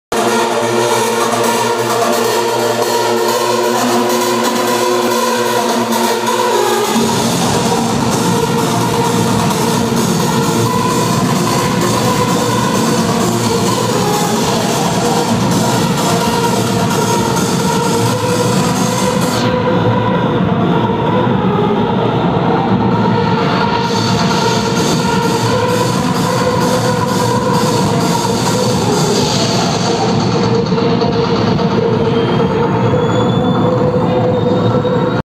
Noize Faktory 3-9-2014
Just some short video of Noize Faktory in Los Angeles on 3/9/2014.  DJ Trauma spinning, some fire dancers, DJ Franck H-Bomb on stage relaxing, and some of the crowd.